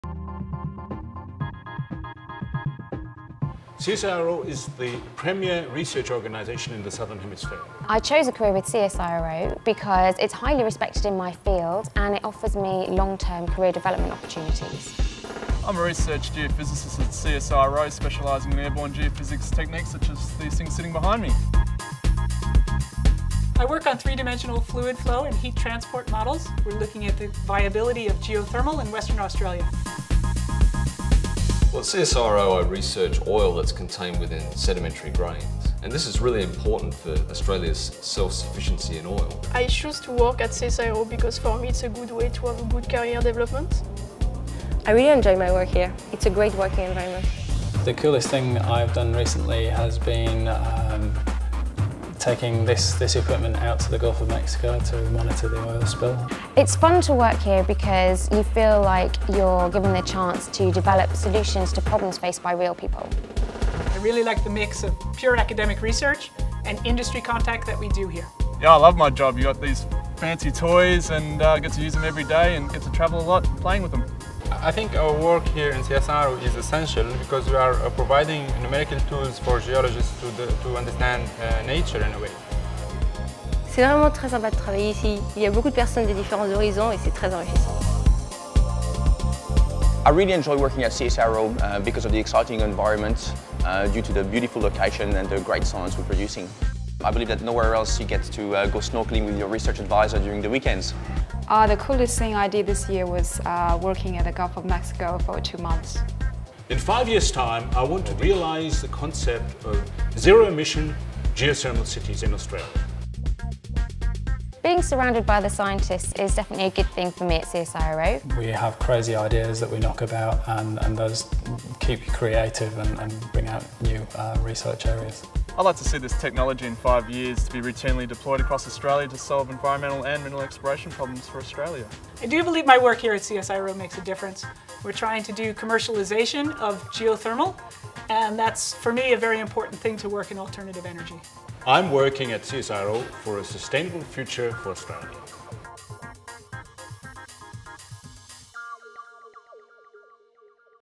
What's it like working as a scientist?
We aim to deliver great science and innovative solutions for industry, society and the environment. We know It's not enough just to have a great idea. At our Earth Science & Resource Engineering Australian Resources Research Centre, we want to have an impact, solve problems and make a difference. Great science is our foundation. And getting it out there's our aim.